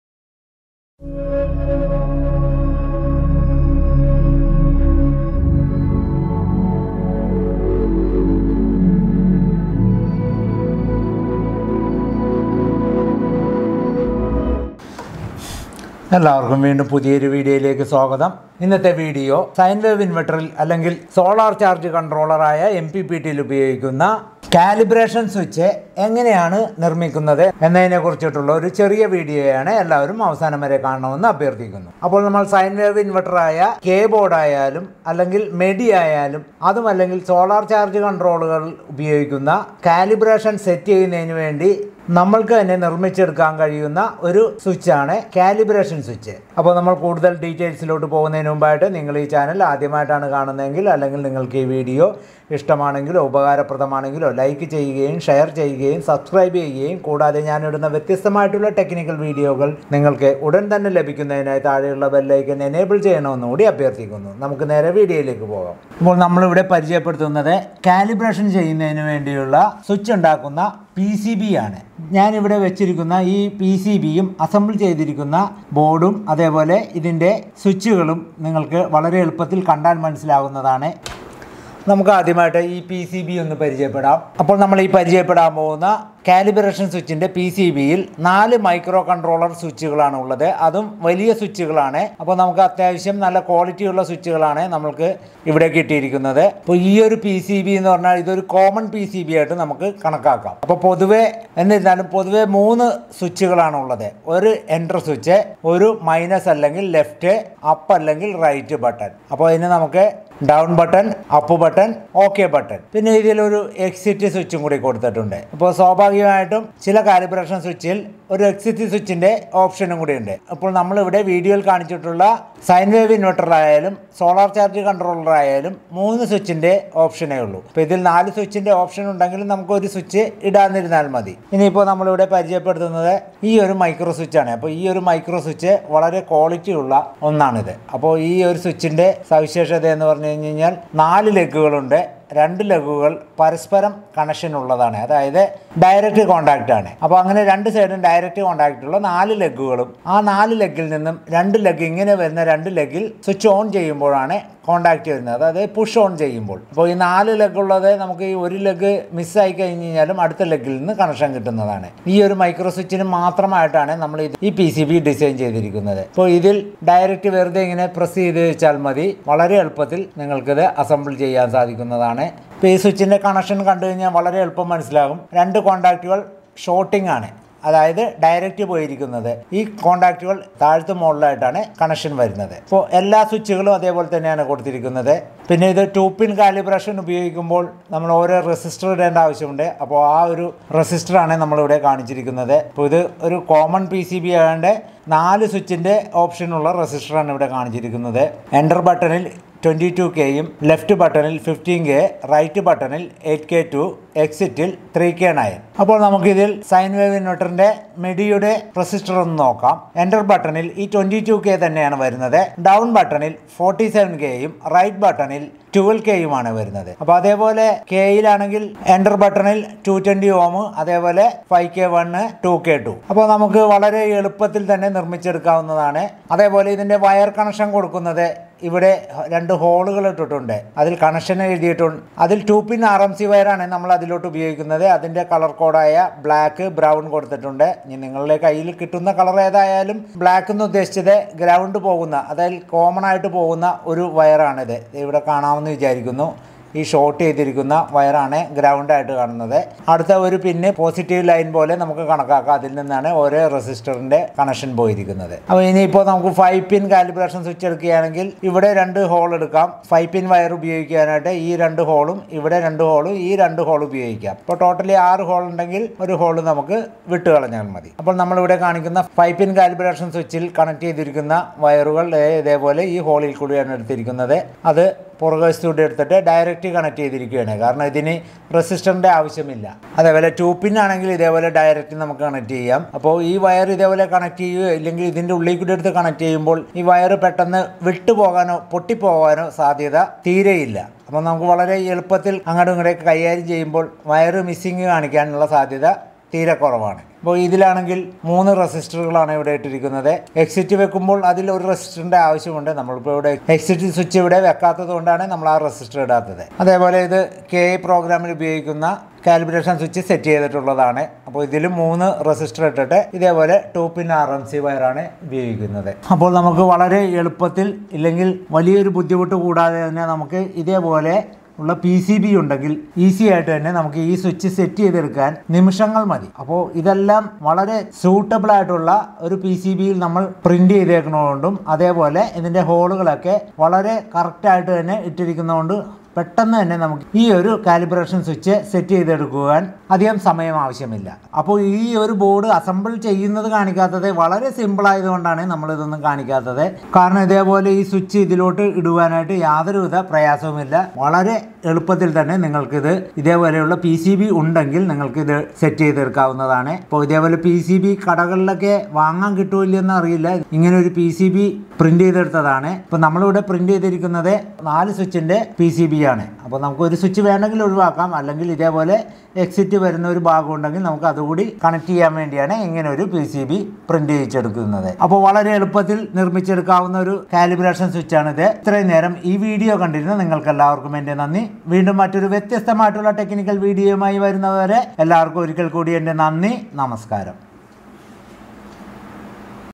How to assemble calibration switch with exact pcb I inverter & mppt I the cute pcb
How to assemble calibration switch with exact pcb. The original pcb for calibration switch.  NOTE : All Content used is copyright to Walk With ELECTRICAL AND PLUMBING WORKS B-CLASS.